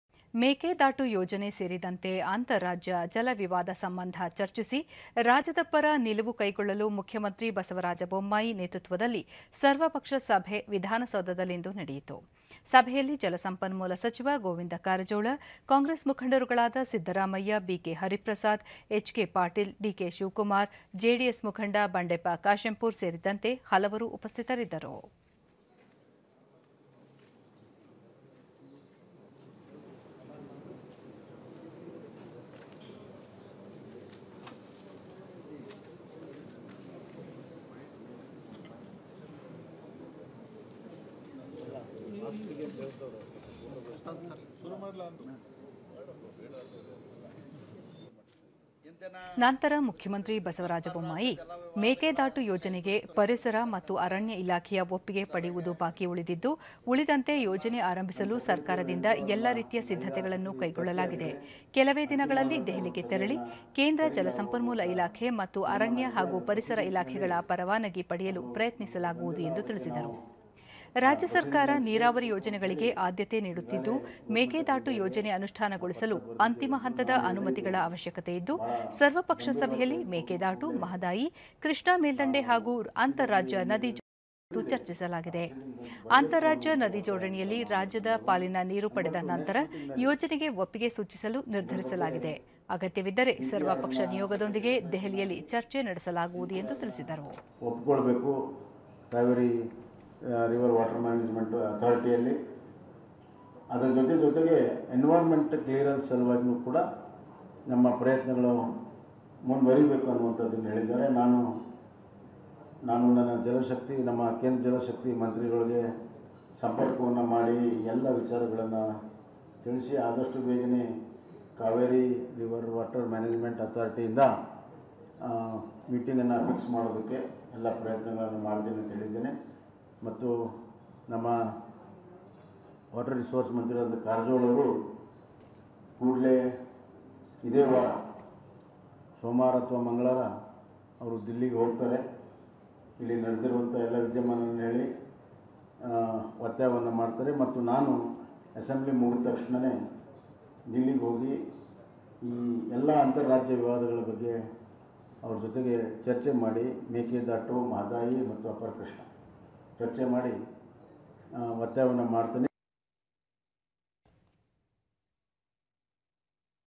ಮೇಕೆದಾಟು ಯೋಜನೆ ಅಂತಾರಾಜ್ಯ ಜಲವಿವಾದ ಸಂಬಂಧ ಮುಖ್ಯಮಂತ್ರಿ ಬಸವರಾಜ ಬೊಮ್ಮಾಯಿ ನೇತೃತ್ವದಲ್ಲಿ ಸರ್ವಪಕ್ಷ ಸಭೆ
ಮೇಕೆದಾಟು ಯೋಜನೆ ಸೇರಿದಂತೆ ಅಂತಾರಾಜ್ಯ ಜಲವಿವಾದ ಸಂಬಂಧ ಚರ್ಚಿಸಿ, ರಾಜ್ಯದ ಪರ ನಿಲುವು ಕೈಗೊಳ್ಳಲು ಮುಖ್ಯಮಂತ್ರಿ ಬಸವರಾಜ ಬೊಮ್ಮಾಯಿ ನೇತೃತ್ವದಲ್ಲಿ ಸರ್ವಪಕ್ಷ ಸಭೆ ವಿಧಾನಸೌಧದಲ್ಲಿಂದು ನಡೆಯಿತು.
ಸಭೆಯಲ್ಲಿ ಜಲಸಂಪನ್ಮೂಲ ಸಚಿವ ಗೋವಿಂದ ಕಾರಜೋಳ, ಕಾಂಗ್ರೆಸ್ ಮುಖಂಡರುಗಳಾದ  ಸಿದ್ದರಾಮಯ್ಯ, ಬಿ.ಕೆ. ಹರಿಪ್ರಸಾದ್, ಎಚ್.ಕೆ. ಪಾಟೀಲ್, ಡಿ.ಕೆ. ಶಿವಕುಮಾರ್, ಜೆಡಿಎಸ್ ಮುಖಂಡ ಬಂಡೆಪ್ಪ ಕಾಶ್ಯಂಪೂರ್ ಸೇರಿದಂತೆ ಹಲವರು ಉಪಸ್ಥಿತರಿದ್ದರು.

ನಂತರ ಮುಖ್ಯಮಂತ್ರಿ ಬಸವರಾಜ ಬೊಮ್ಮಾಯಿ, ಮೇಕೆದಾಟು ಯೋಜನೆಗೆ ಪರಿಸರ ಮತ್ತು ಅರಣ್ಯ ಇಲಾಖೆಯ ಒಪ್ಪಿಗೆ ಪಡೆಯುವುದು ಬಾಕಿ ಉಳಿದಿದ್ದು, ಉಳಿದಂತೆ ಯೋಜನೆ ಆರಂಭಿಸಲು ಸರ್ಕಾರದಿಂದ ಎಲ್ಲಾ ರೀತಿಯ ಸಿದ್ಧತೆಗಳನ್ನು ಕೈಗೊಳ್ಳಲಾಗಿದೆ. ಕೆಲವೇ ದಿನಗಳಲ್ಲಿ ದೆಹಲಿಗೆ ತೆರಳಿ, ಕೇಂದ್ರ ಜಲಸಂಪನ್ಮೂಲ ಇಲಾಖೆ ಮತ್ತು ಅರಣ್ಯ ಹಾಗೂ ಪರಿಸರ ಇಲಾಖೆಗಳ ಪರವಾನಗಿ ಪಡೆಯಲು ಪ್ರಯತ್ನಿಸಲಾಗುವುದು ಎಂದು ತಿಳಿಸಿದರು.
ರಾಜ್ಯ ಸರ್ಕಾರ ನೀರಾವರಿ ಯೋಜನೆಗಳಿಗೆ ಆದ್ಯತೆ ನೀಡುತ್ತಿದ್ದು, ಮೇಕೆದಾಟು ಯೋಜನೆ ಅನುಷ್ಠಾನಗೊಳಿಸಲು ಅಂತಿಮ ಹಂತದ ಅನಮತಿಗಳ ಅವಶ್ಯಕತೆಯಿದ್ದು, ಸರ್ವಪಕ್ಷ ಸಭೆಯಲ್ಲಿ ಮೇಕೆದಾಟು, ಮಹದಾಯಿ, ಕೃಷ್ಣಾ ಮೇಲ್ದಂಡೆ ಹಾಗೂ ಅಂತಾರಾಜ್ಯ ನದಿ ಜೋಡಣೆ ಕುರಿತು ಚರ್ಚಿಸಲಾಗಿದೆ. ಅಂತಾರಾಜ್ಯ ನದಿ ಜೋಡಣೆಯಲ್ಲಿ ರಾಜ್ಯದ ಪಾಲಿನ ನೀರು ಪಡೆದ ನಂತರ, ಯೋಜನೆಗೆ ಒಪ್ಪಿಗೆ ಸೂಚಿಸಲು ನಿರ್ಧರಿಸಲಾಗಿದೆ. ಅಗತ್ಯವಿದ್ದರೆ, ಸರ್ವಪಕ್ಷ ನಿಯೋಗದೊಂದಿಗೆ ದೆಹಲಿಯಲ್ಲಿ ಚರ್ಚೆ ನಡೆಸಲಾಗುವುದು ಎಂದು ತಿಳಿಸಿದರು.